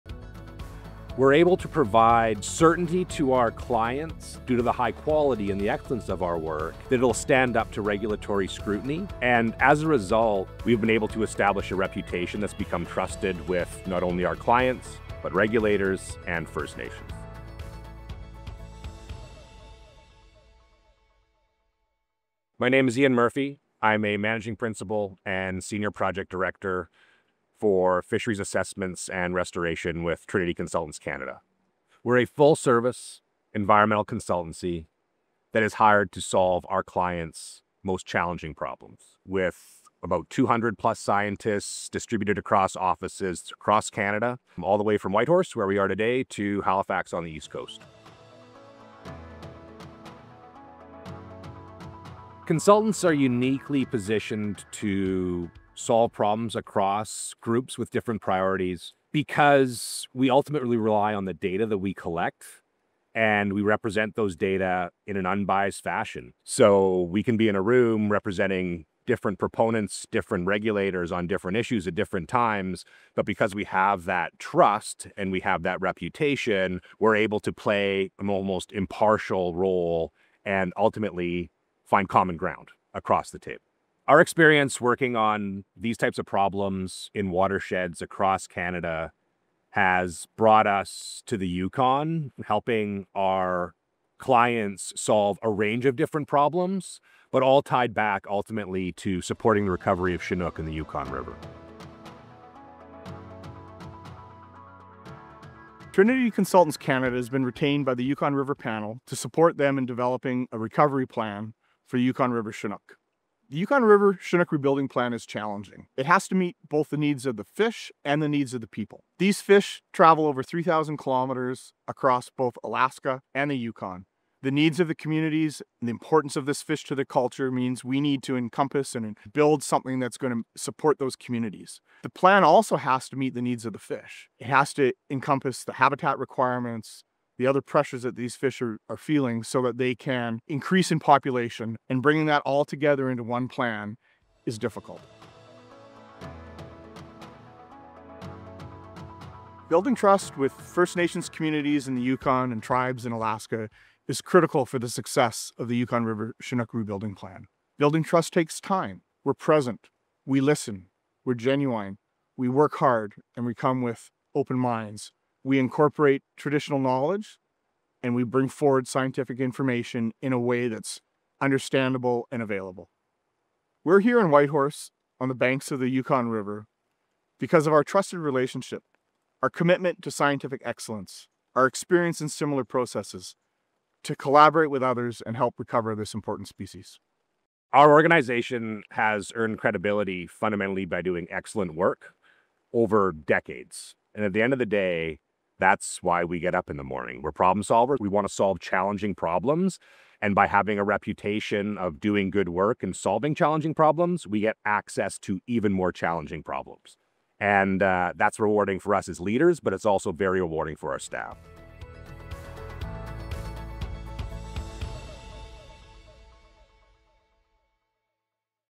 Solving Complex Environmental Challenges | Trinity Consultants Canada | Tomorrow's Catch
Meet Ian Murphy, Managing Principal and Senior Project Director at Trinity Consultants Canada, as he leads efforts to recover Yukon River Chinook salmon. With decades of experience in fisheries assessment and restoration, Ian and his team are helping develop a collaborative rebuilding plan rooted in scientific excellence, traditional knowledge, and trust with First Nations. Filmed on the banks of the Yukon River, this story highlights the critical role of environmental consultants in supporting both ecosystems and communities.

Learn more about the series here: contentwithpurpose.co.uk/afs/tomorrowscatch